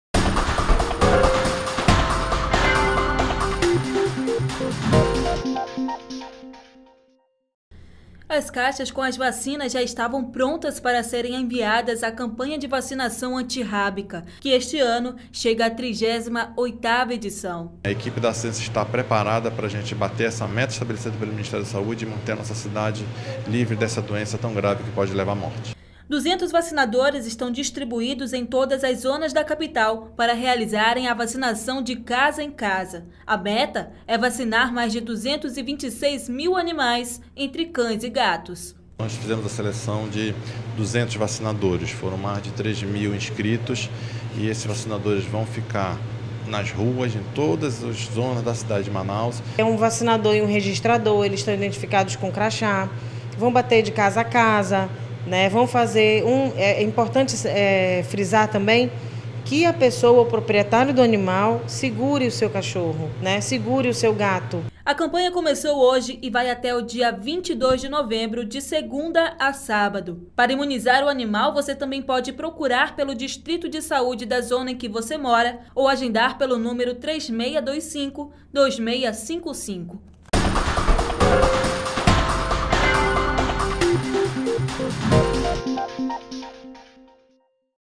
Começa campanha de vacinação antirrábica em cães e gatos na capital
Começou nesta quarta-feira, 4, a campanha de vacinação antirrábica da Prefeitura de Manaus. Mais de 220 mil animais, entre cães e gatos, devem ser imunizados.